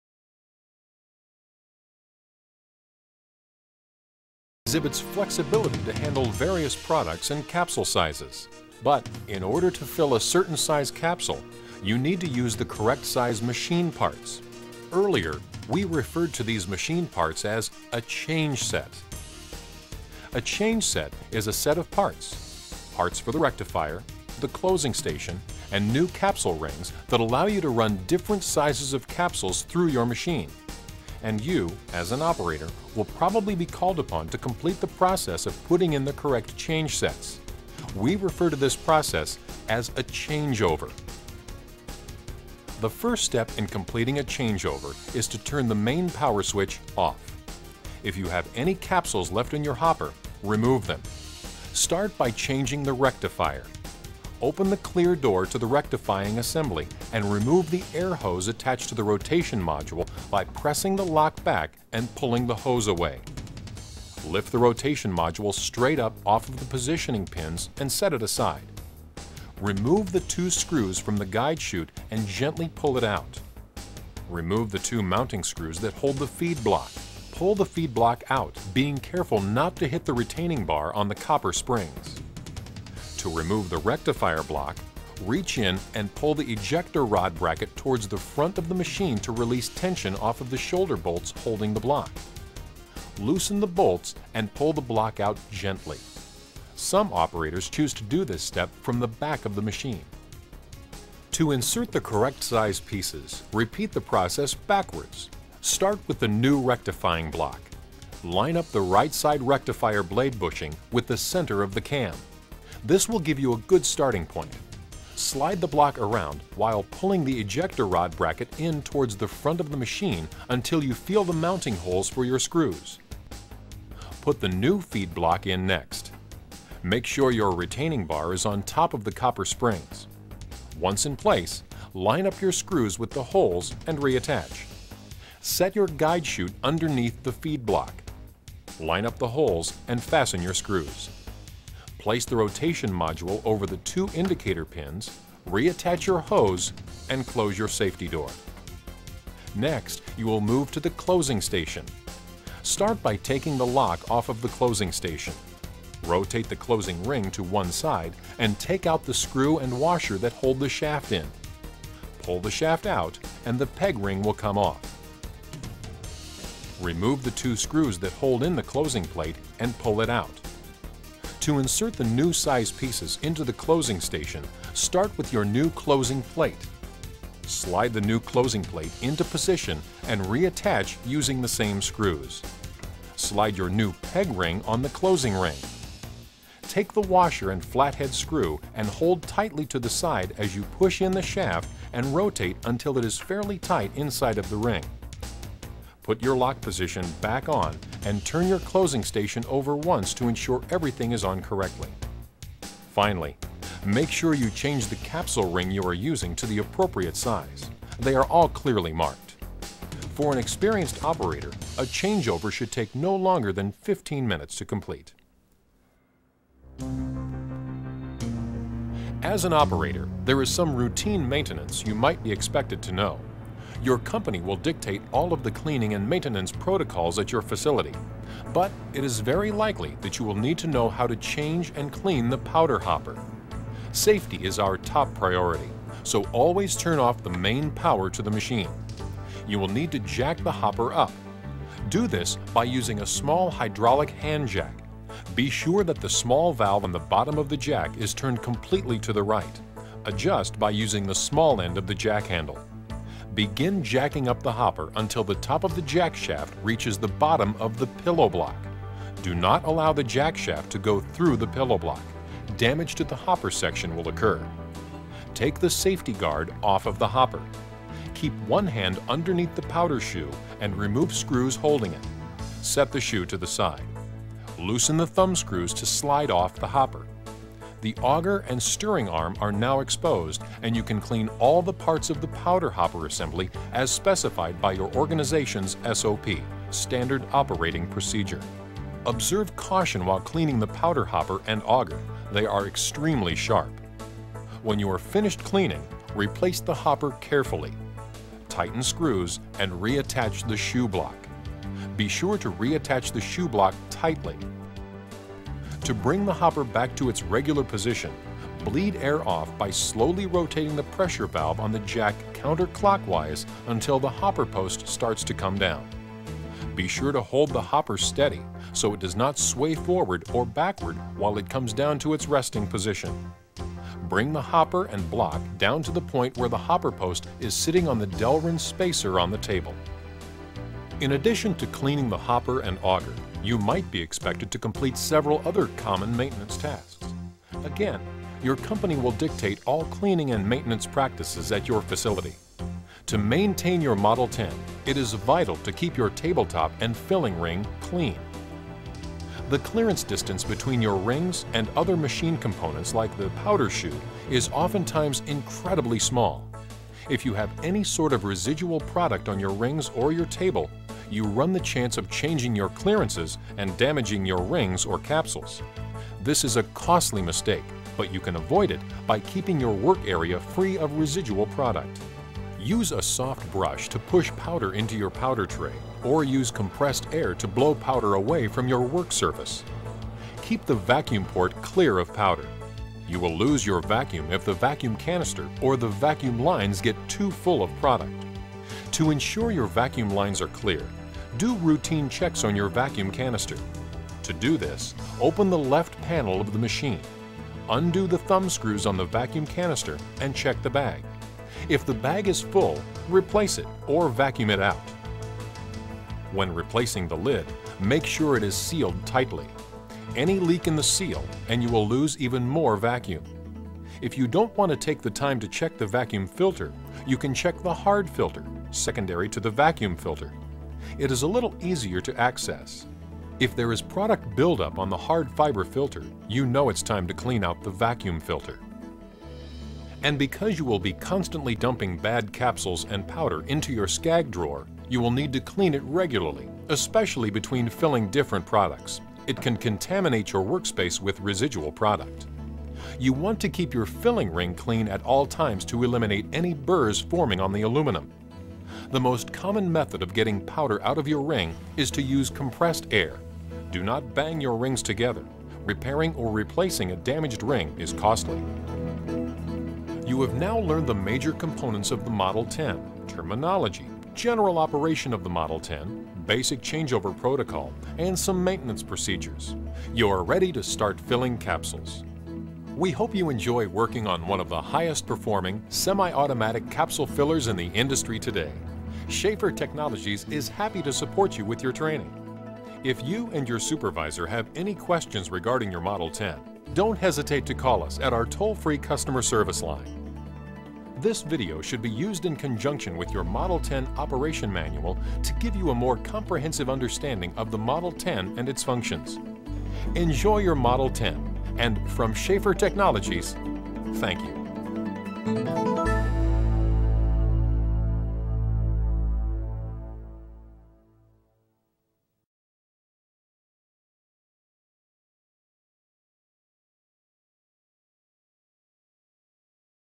STI Model10 ver. 2 Operation Part 2
Schaefer Technologies, Inc. - STI Model 10 Semi - Automatic Capsule Filling Machine for powder and pellets.